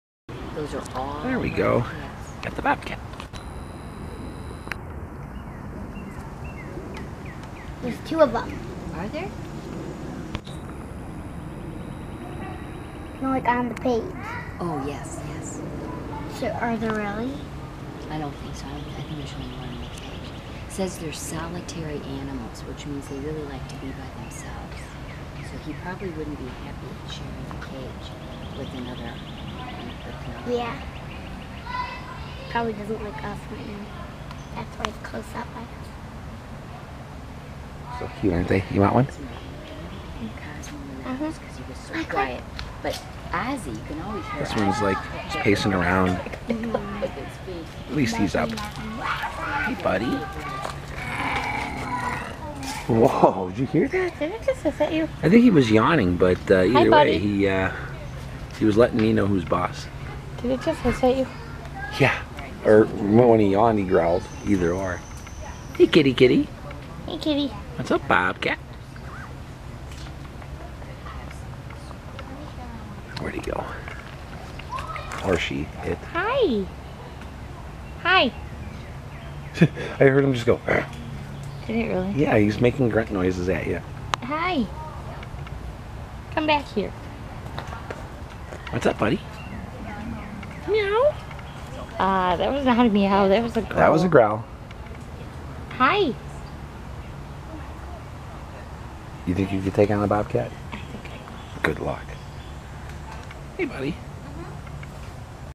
Bobcat Willowbrook Wildlife Center Glen Ellyn, IL 7-26-17
L. Michelle and I got to see a bobcat up and around! It was even purring and grunting!